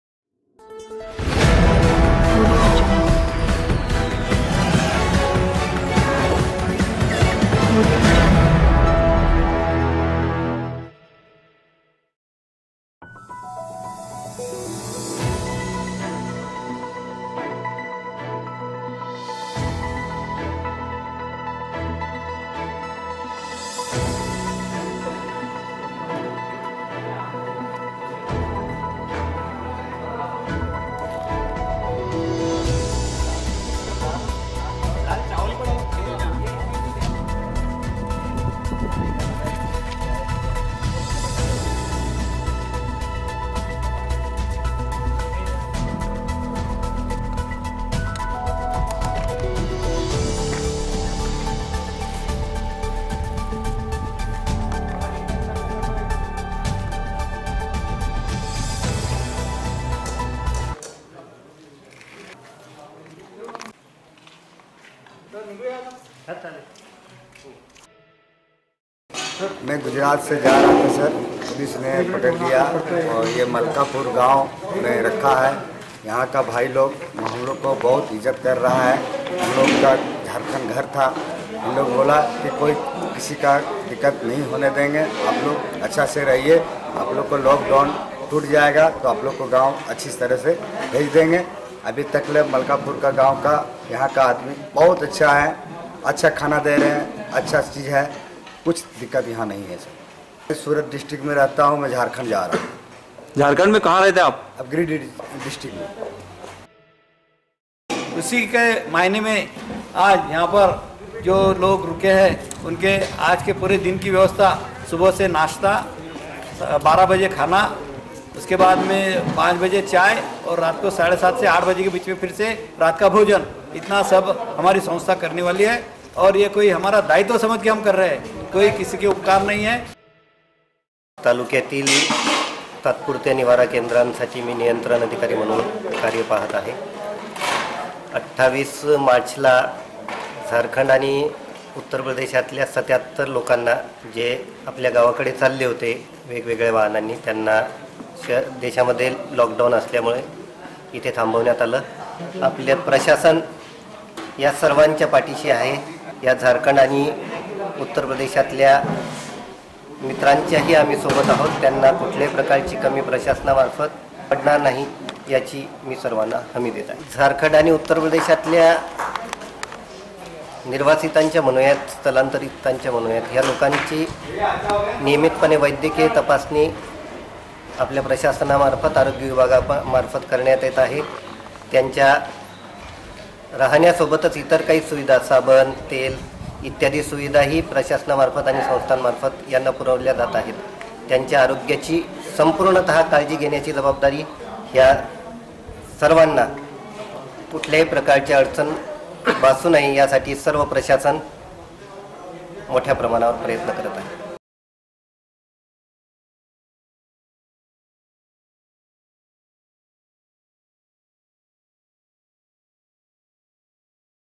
मलकापूर : ७६ परप्रांतीय मजुर अडकले
कि. संचेती पतसंस्थेच्यावतीने करण्यात आली भोजनाची व्यवस्था